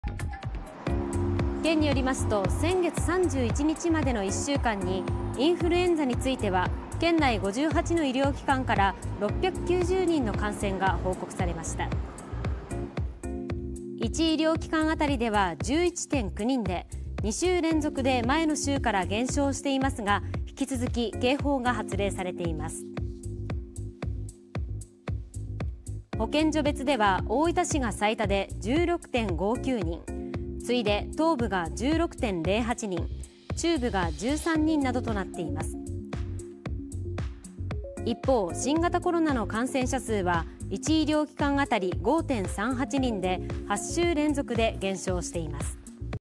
インフルエンザ・新型コロナともに減少も インフルエンザは警報発令中 大分
インフルエンザや新型コロナについて大分県内の最新の動向が3日、発表されました。
インフルエンザは引き続き警報が発令されています。

県によりますと、3月31日までの1週間に、インフルエンザについては県内58の医療機関から690人の感染が報告されました。

1医療機関あたりでは11.9人で、2週連続で前の週から減少していますが、引き続き警報が発令されています。

保健所別では、大分市が最多で16.59人、次いで東部が16.08人中部が13人などとなっています。

一方、新型コロナの感染者数は、1医療機関あたり5.38人で8週連続で減少しています。